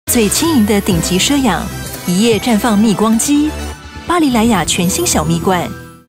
全新 小蜜罐 系列 最輕盈的頂級奢養 ✨一夜綻放緊彈蜜光肌
風靡小紅書🔥巴黎萊雅全新 小蜜罐 系列來了📣
最輕盈的頂級奢養 ✨一夜綻放緊彈蜜光肌
三大頂級萬元乳霜成分🔥
珍稀麥盧卡花蜜🟡滋養透亮
高濃度普拉斯鏈🟡修護撫紋
黃金四胜肽精萃🟡緊緻彈潤